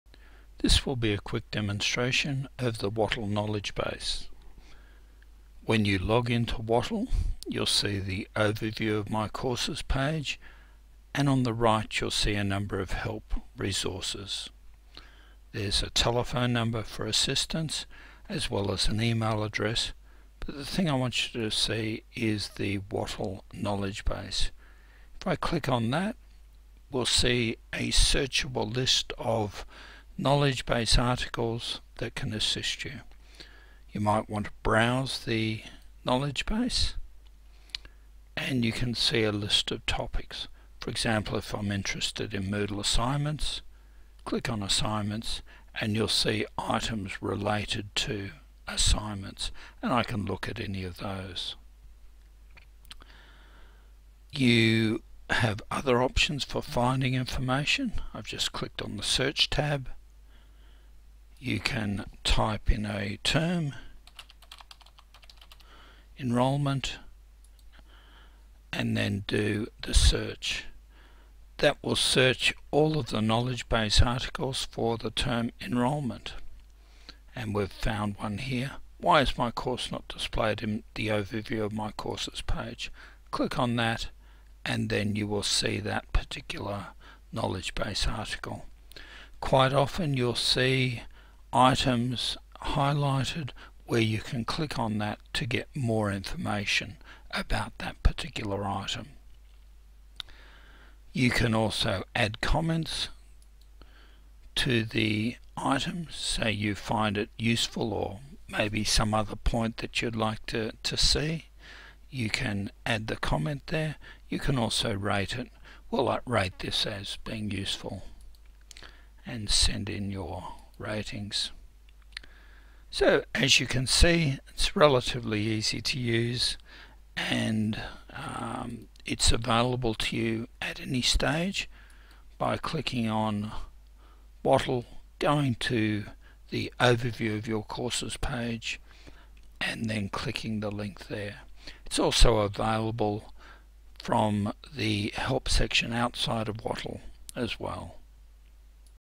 Screencast by Wattle_Support from Screenr.com
Overview of the Wattle knowledgebase allowing staff and students to search for information on using Wattle.